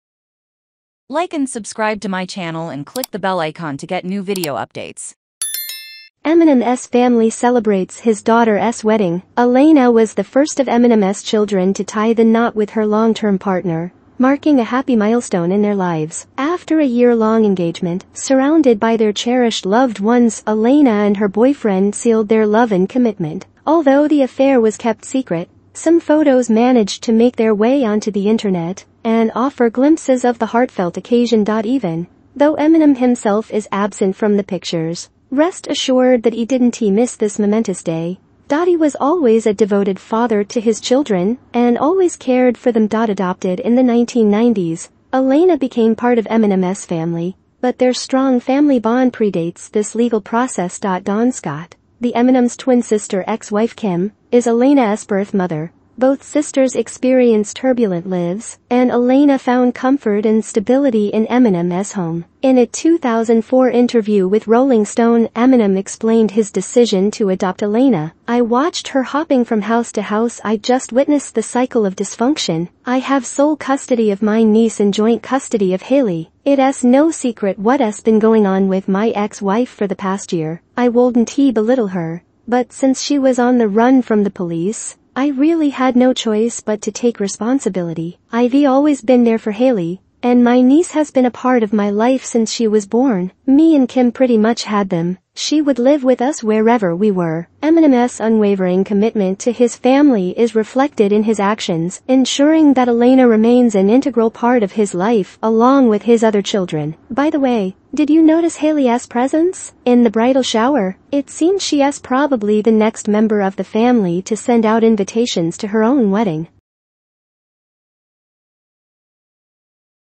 EMINEMS FAMILY CELEBRATES HIS DAUGHTERS WEDDING
Alaina was the first of Eminems children to tie the knot with her longtime partner. marking a joyful milestone in her life. I have sole custody of my niece and joint custody of Hailie.